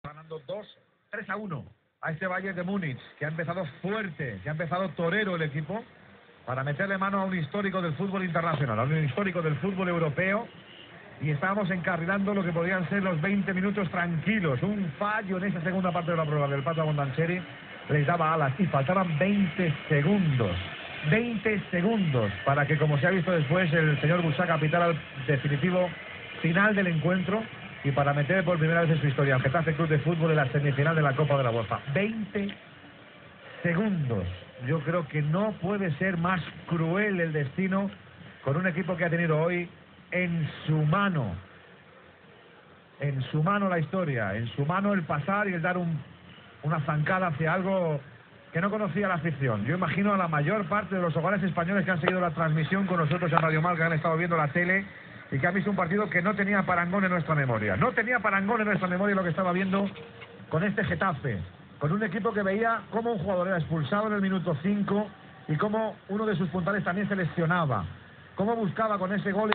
Nueva Radio Marca por 107.6 FM desde Sevilla con RDS
Ya es oficial, ya hay una nueva emisora de Radio Marca en Sevilla o provincia, por 107.6 FM y hasta con RDS, en la franja de la Banda de FM reservada a las emisoras municipales.

10/04/2008